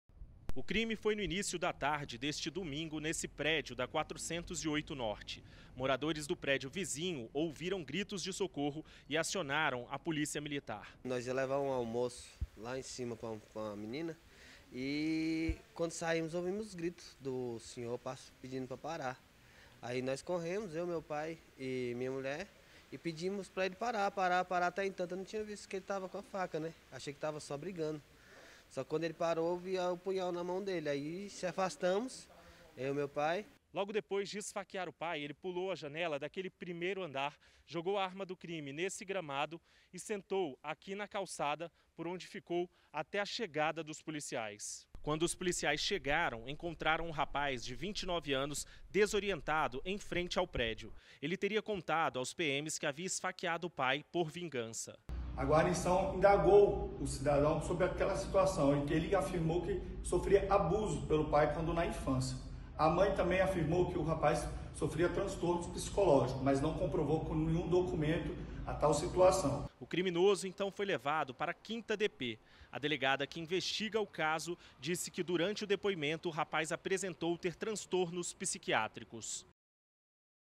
FILHO ESFAQUEIA O PRÓPRIO PAI NA ASA NORTE | Band Brasília 21/02/2022
UM HOMEM ESFAQUEOU E MATOU O PRÓPRIO PAI NA ASA NORTE NESTE DOMINGO. SEGUNDO O RAPAZ, ELE SOFRIA ABUSOS DO PAI QUANDO ERA CRIANÇA. CONFIRA OS DETALHES NA REPORTAGEM DE ANDRÉ LAGE.